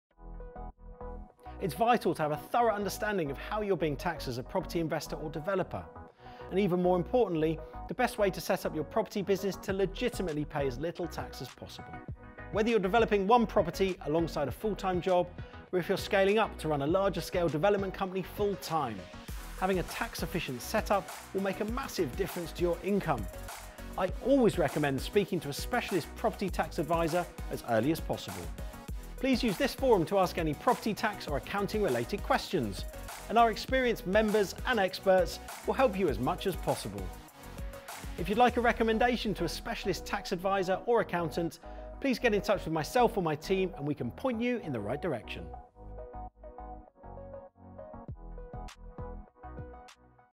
Property Tax & Accounting - Develop Understanding How To Legitimately Pay As Little Tax As Possible.
It’s vital to have a thorough understanding on how you are being taxed as an investor or developer, and how to ensure that you are legitimately paying as little tax as possible. Whether you are developing one property or scaling up a development portfolio, having a tax efficient set-up will have a massive impact on your income. Use our specialist forum to ask any property tax or accounting related questions and our experts and 65,000+ members will help you as much as possible.